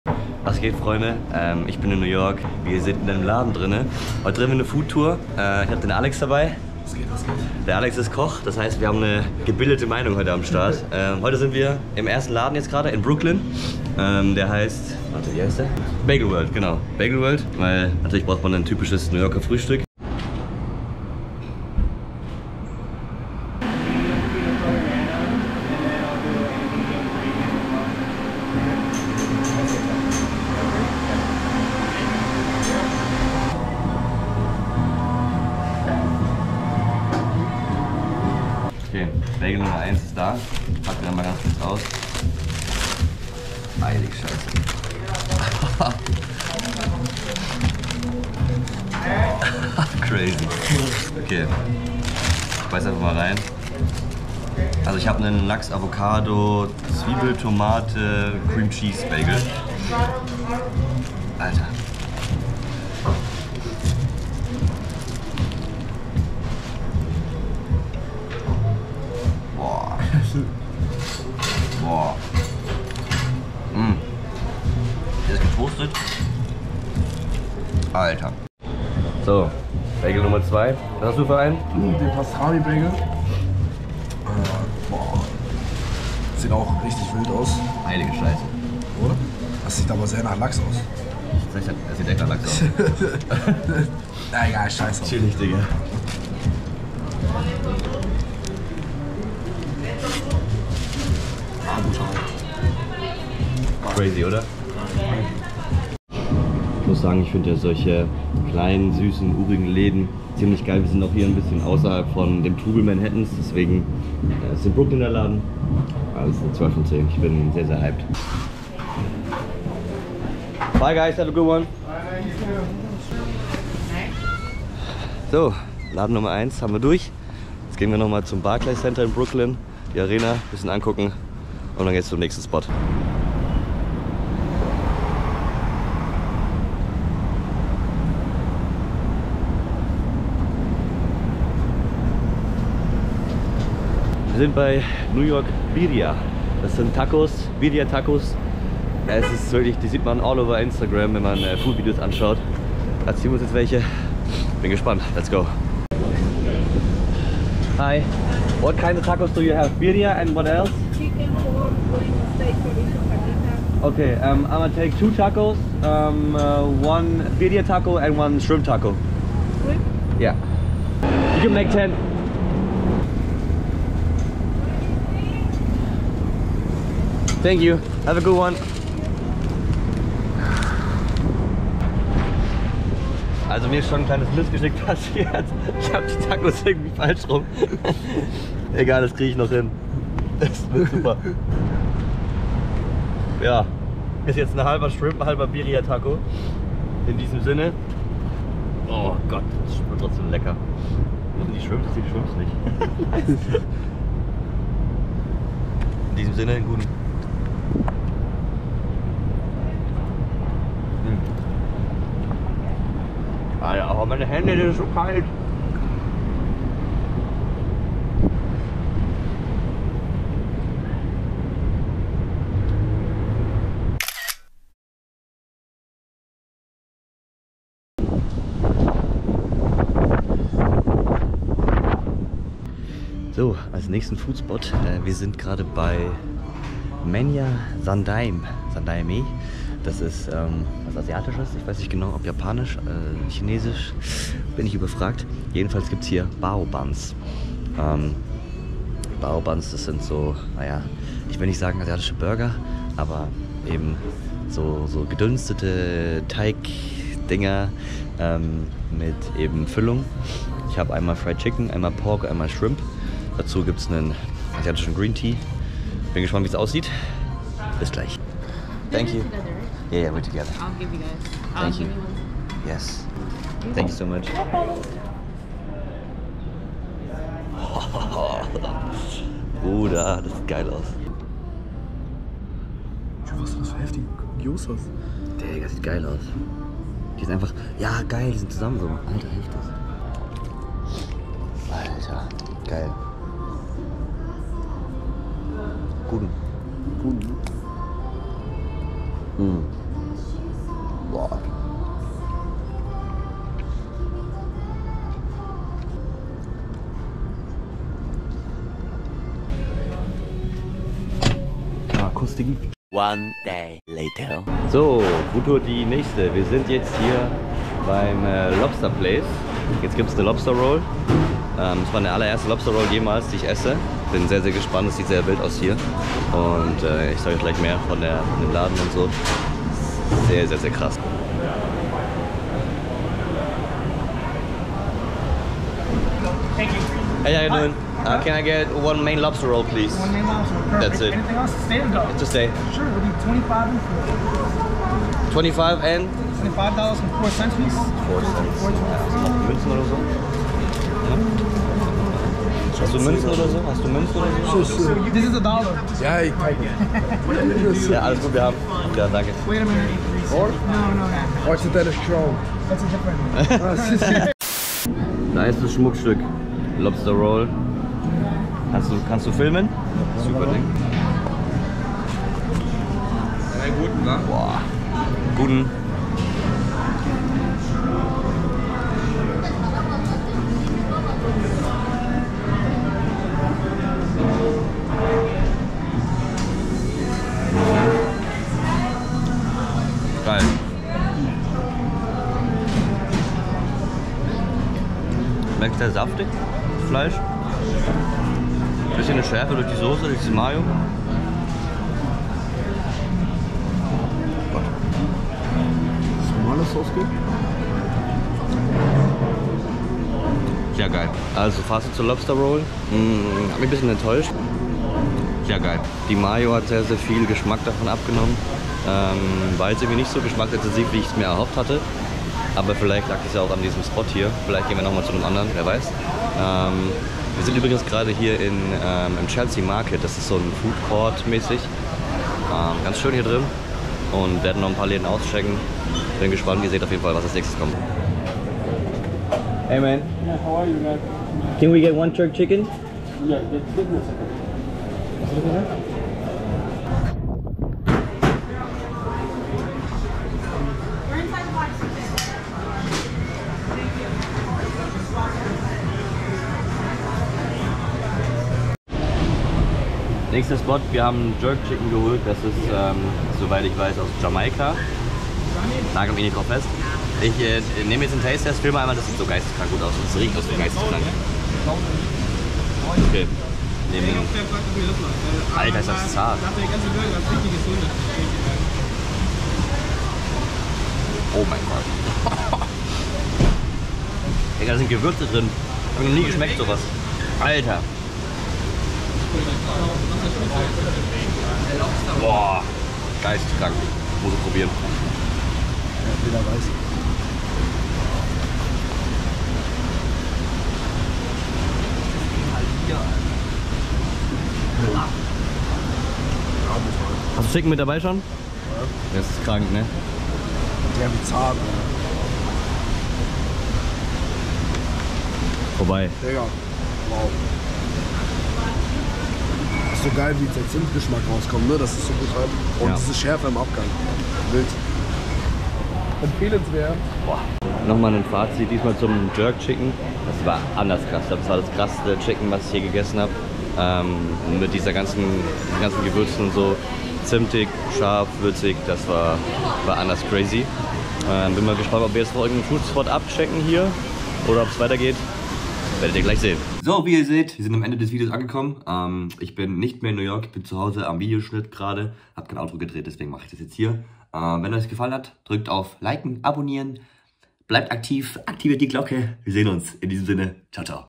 New York City FOODTOUR!! 🇺🇸🇺🇸 (Lobster, Bagel, etc...)
💥 Ich war in NYC & habe eine Foodtour gedreht! Viel Spaß!

Hi ich bin Jonas & ich versuche auf diesem Kanal einfach nur lustig zu sein.
Wenn euch gefällt was ich mache, yallah rein ins Abo!